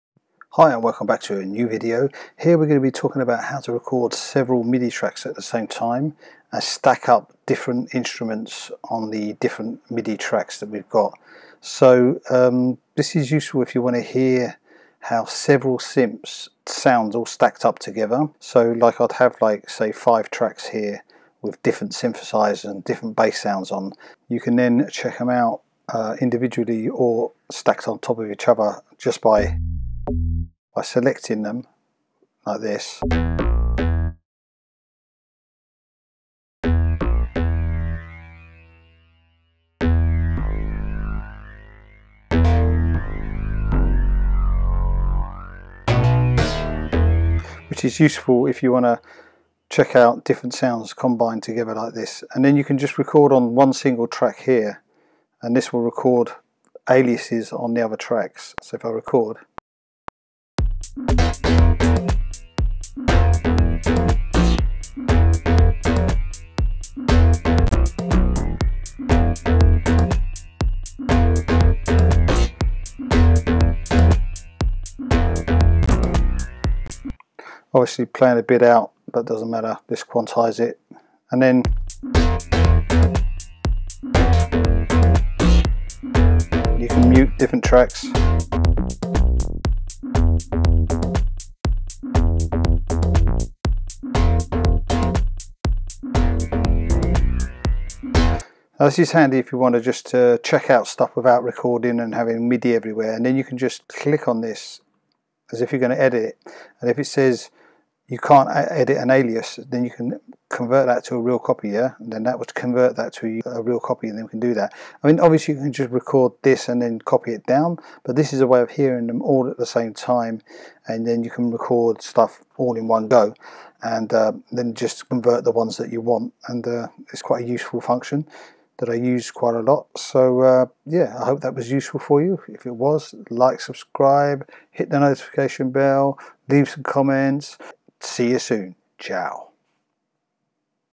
HOW TO RECORD MULTI TRACKS : Quick Tip LOGIC PRO X
Here i show you how to listen to and record multi midi tracks at the same time .